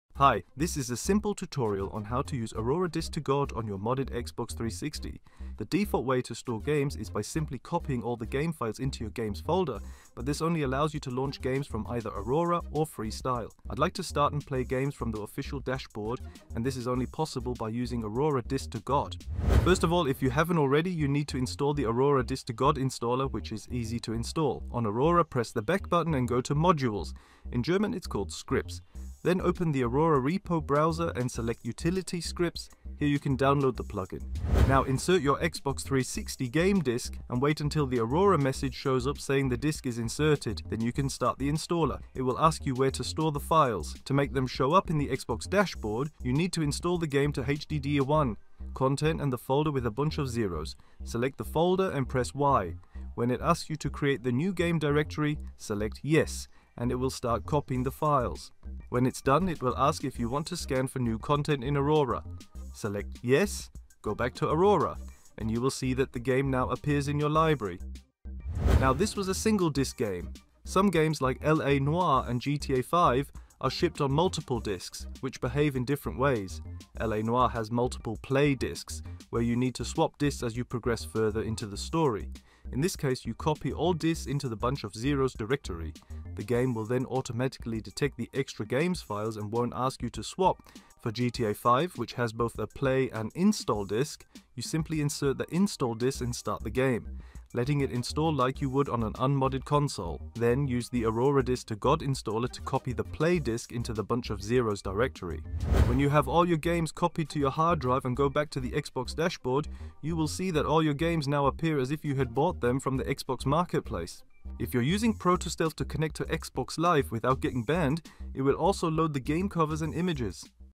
Aurora Disc to GOD | Start Ripped Games from the Dashboard!
Install and use Aurora Disc to GOD to play ripped Xbox 360 games from the dashboard. This tutorial covers installation, copying single and multi-disc games, and making your games appear as if bought from the Xbox Marketplace.